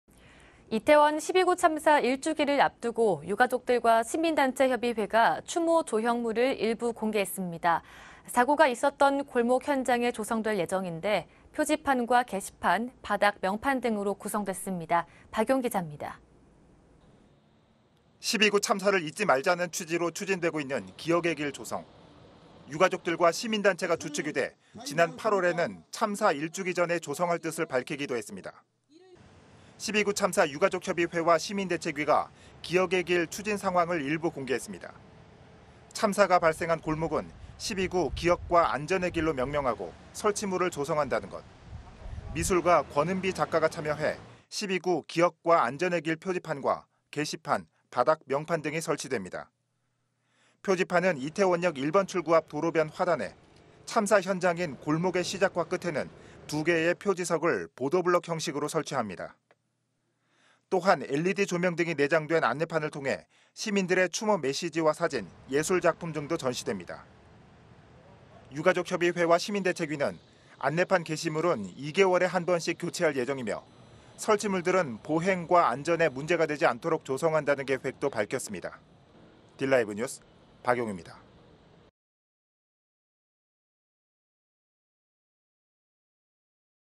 10.29 참사 골목에 추모설치물 조성 [용산] 딜라이브TV
진행 : 백시연 아나운서

【 앵커멘트 】
이태원 10.29참사 1주기를 앞두고
유가족들과 시민단체협의회가
추모 조형물을 일부 공개했습니다.
사고가 있었던 골목 현장에
조성될 예정인데
표지판과 게시판,
바닥 명판 등으로 구성됐습니다.
박용 기자입니다.

【 VCR 】
10.29 참사를 잊지 말자는 취지로
추진되고 있는 '기억의 길' 조성.

유가족들과 시민단체가 주축이 돼
지난 8월에는 참사 1주기 전에
조성할 뜻을 밝히기도 했습니다.

10.29참사 유가족협의회와 시만대책위가
기억의 길 추진 상황을 일부 공개했습니다.

참사가 발생한 골목은
'10.29 기억과 안전의 길'로 명명하고
설치물을 조성한다는 것.

미술가 권은비 작가가 참여해
'10.29 기억과 안전의 길 표지판'과
게시판, 바닥 명판 등이 설치됩니다.

표지판은 이태원역 1번출구 앞
도로변 화단에,

참사 현장인 골목의 시작과 끝에는
두 개의 표지석을 보도블럭 형식으로
설치합니다.

또한 LED 조명등이 내장된 안내판을 통해
시민들의 추모 메시지와 사진,
예술작품 등도 전시됩니다.

유가족협의회와 시민대책위는,

안내판 게시물은 2개월에 한 번씩
교체할 예정이며
설치물들은 보행과 안전에
문제가 되지 않도록 조성한다는 계획도
밝혔습니다.

딜라이브뉴스 박용입니다.